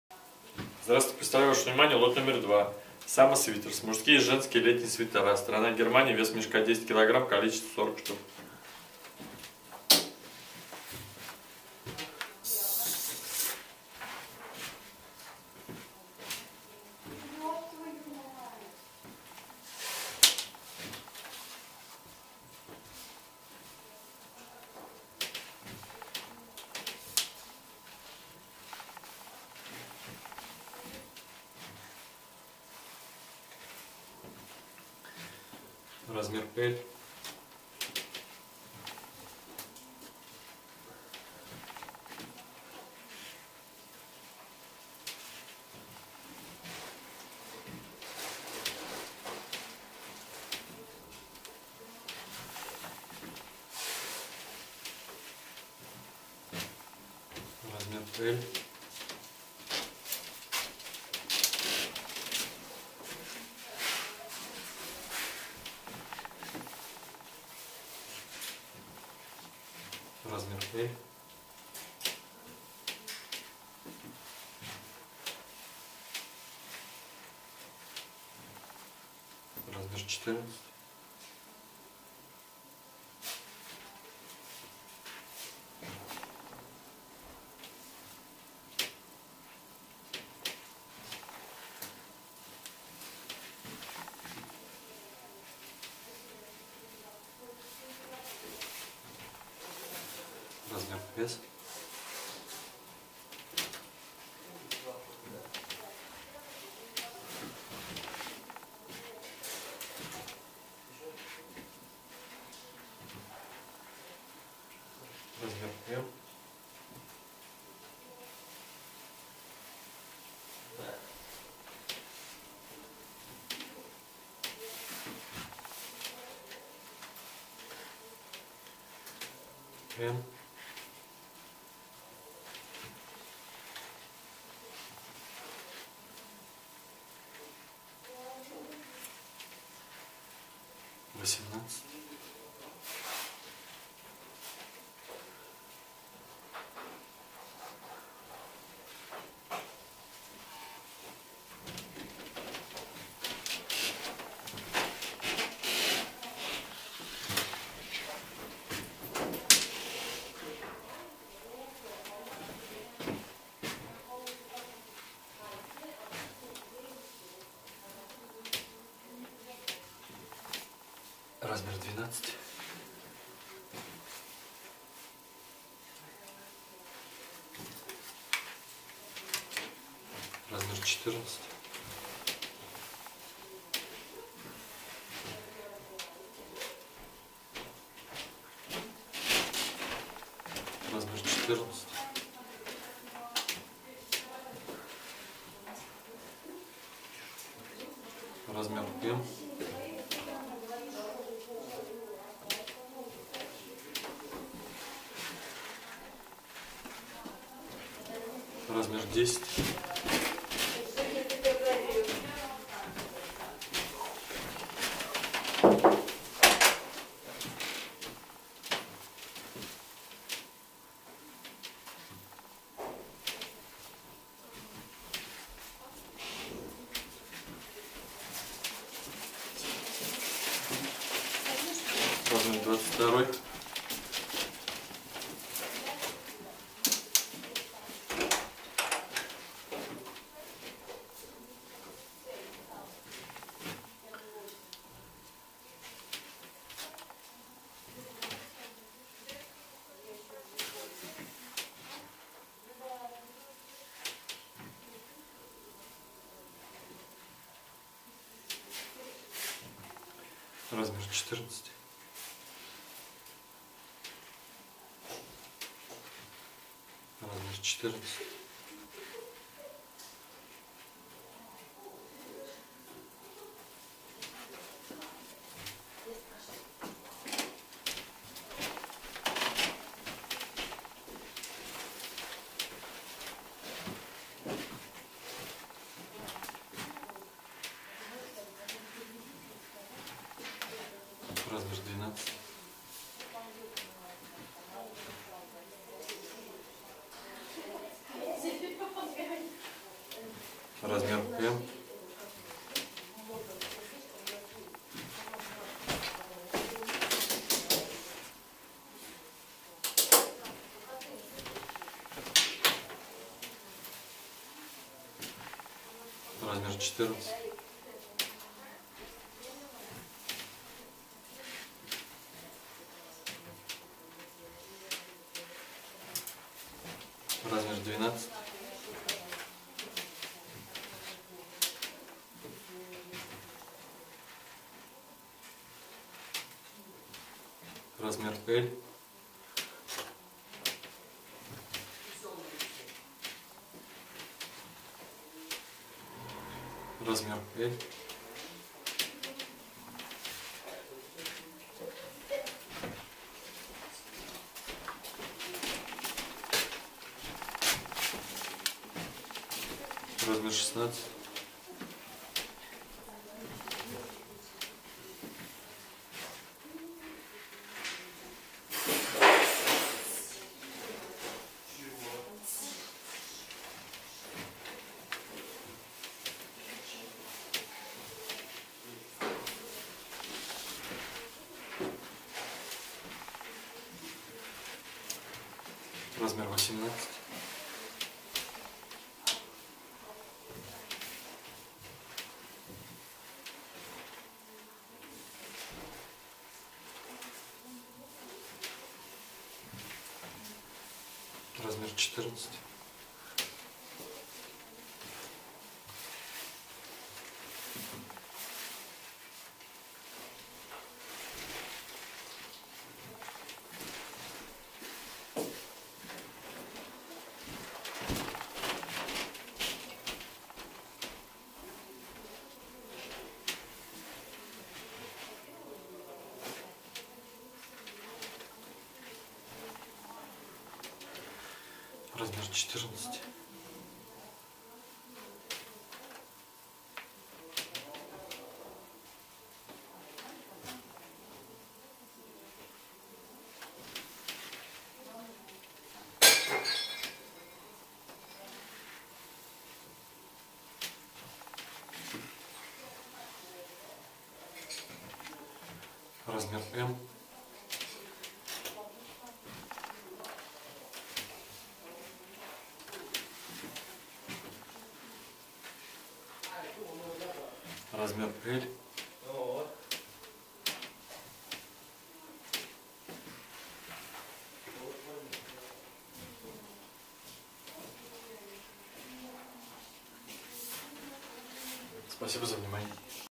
Summer Sweaters 2
Sweaters summer mix (лот №2)
Мужские и женские свитера (секонд хенд)
Страна: Германия
Сезон: Демисезон
Цена за кг.: 5.13
Цена за мешок: 51.3
Цена за ед.: 1.28
Вес мешка, кг.: 10